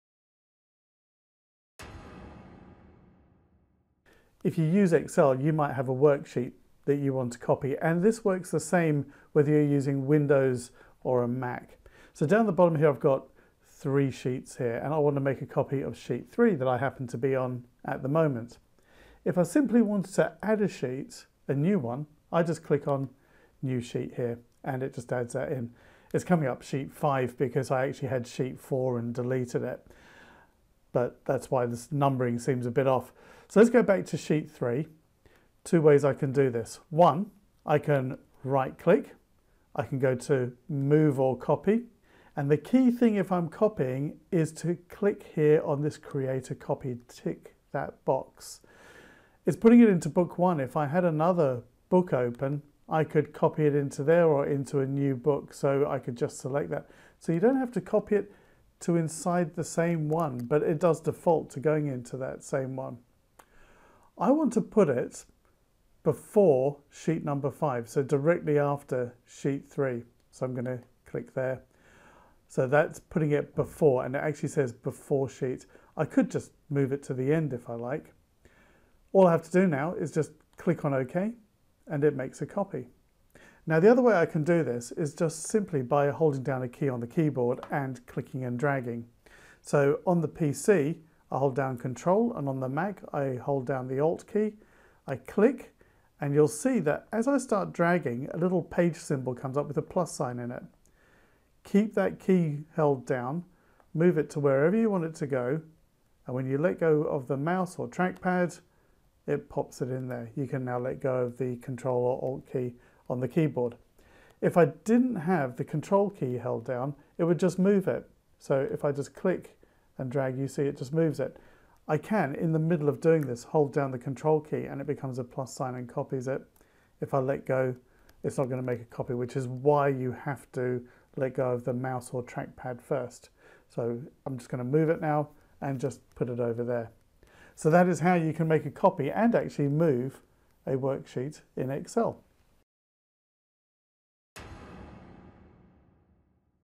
How to Copy an Excel Worksheet
If you need to make a copy of a worksheet in Excel then here are two easy ways to do it. This works for both the Windows and Mac versions of Excel. shows us how.